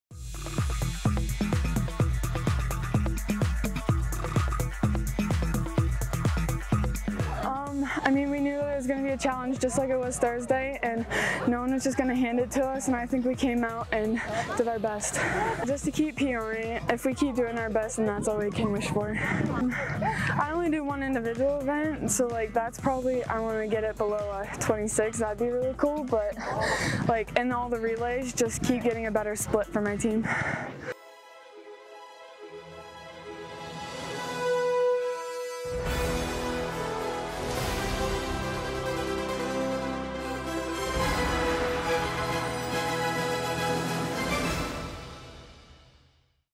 Sophia Gregory Interview
Interview with West Branch's Sophia Gregory. She placed 4th in the girls' 200-meter dash with a time of 26.41. She also ran in the girls' 4x200-meter relay with Lauren Gossett, Zoe Sanders, and Kennedy Berger, placing 2nd with a time of 1:45.39. The same team placed 2nd in the girls' 4x100-meter relay with a time of 49.91 and 3rd in the girls' 4x400-meter relay with a time of 4:08.00. Congratulations Sophia and good luck at regionals!